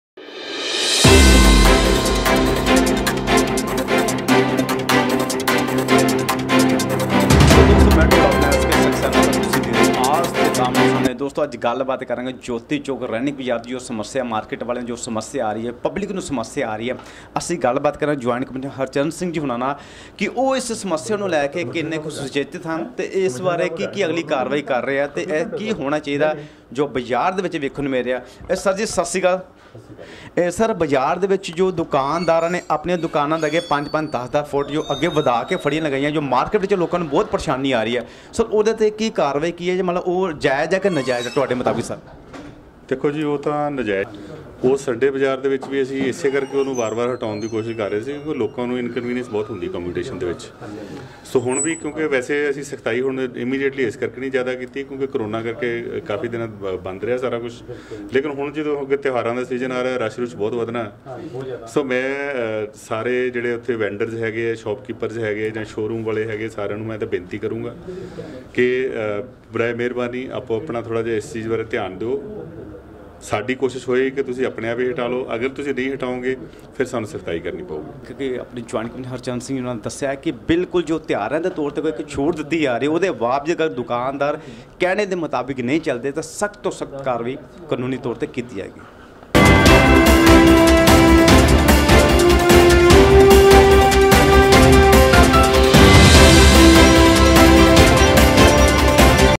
त्योहार तक दी शोरूम और दुकानदारों को शूट -जॉइंट कमिश्नर हरचरण सिंह /Jain Store/AAJ TAK AAMNE SAAMNE
AAMNE SAAMNE GROUP //DIRECTOR SONU
AAJ TAK AAMNE SAAMNE  & -Sabse Fast Aamne }{ Saamne, CONTECT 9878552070 EDITOR SK SAXENA, GM MANDEEP KAUR-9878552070,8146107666